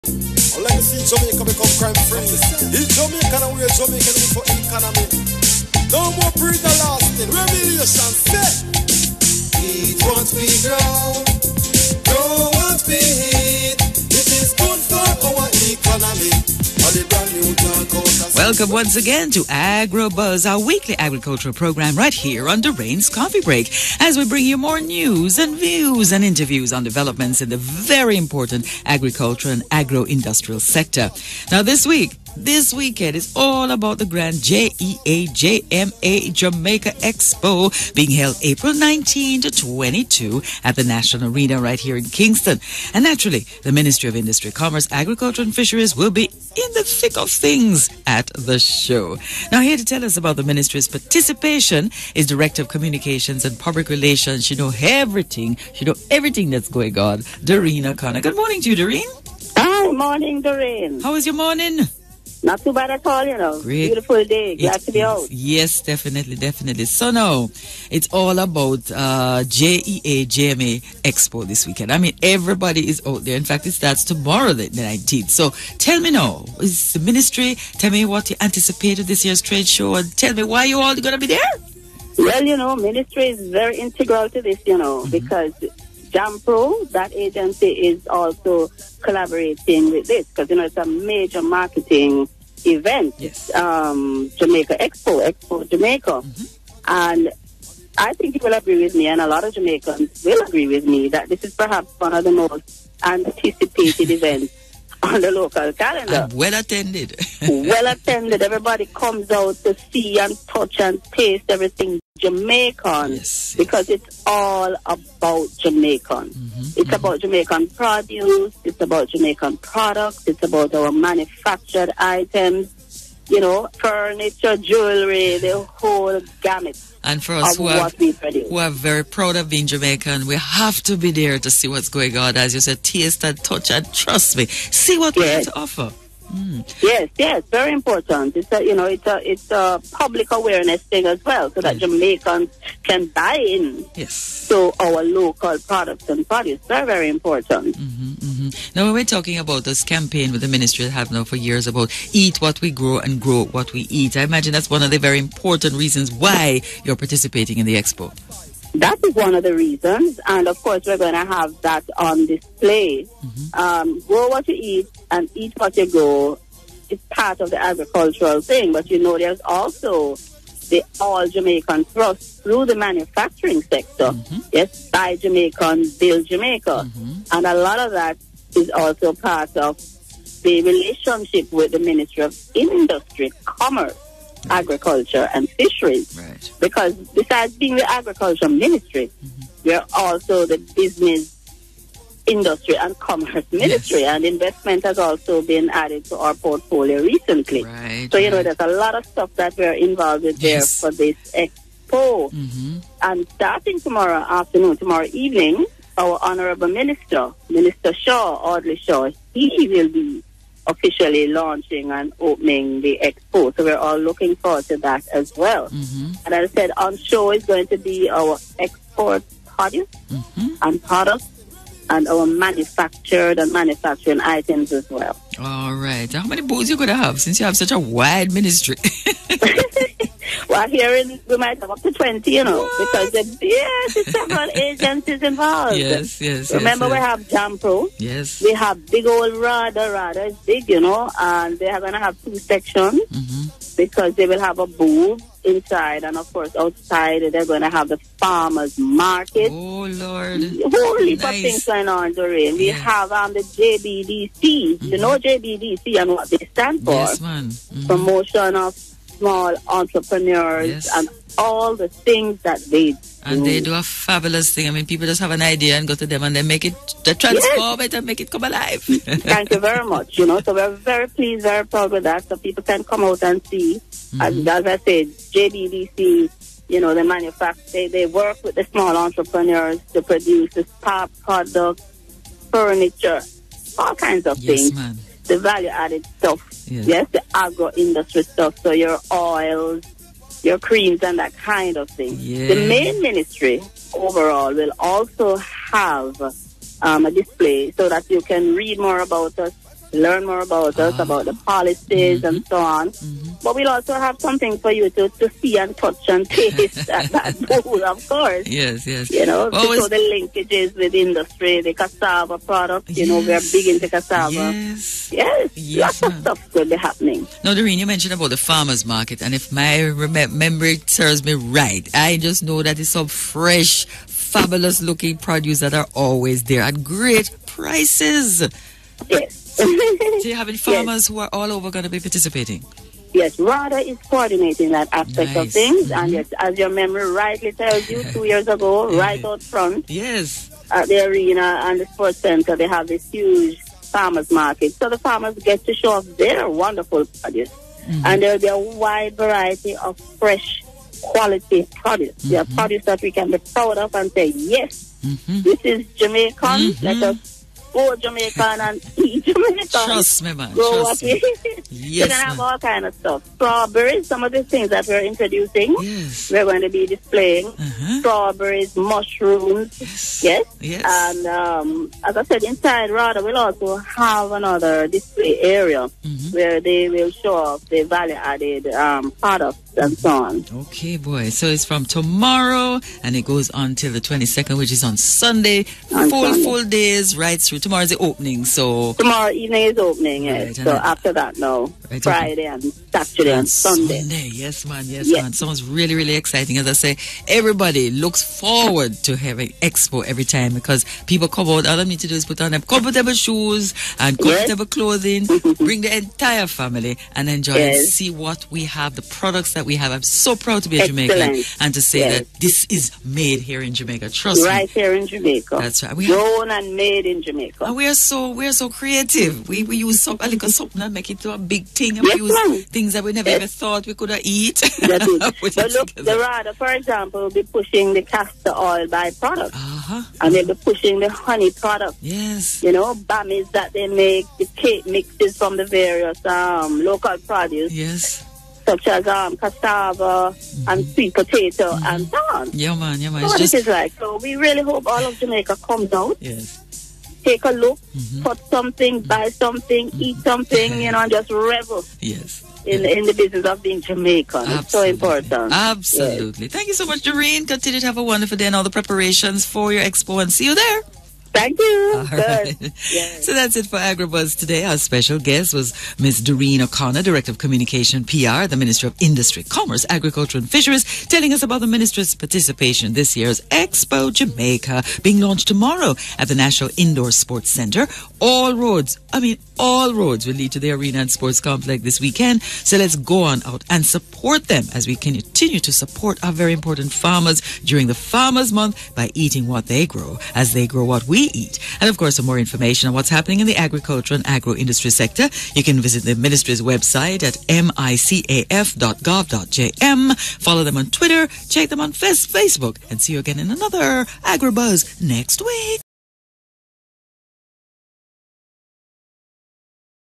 Agrobuzz - JEA/JMA Expo Jamaica - April 18, 2018
Jamaica's premiere trade exposition, 'Expo Jamaica' takes place from April 19-22, 2018 at the National Arena in Kingston. The Expo will feature everything 'Jamaican' from food to furniture, jewellery etc. The Ministry of Industry, Commerce, Agriculture & Fisheries (MICAF) will be showcasing at this expo.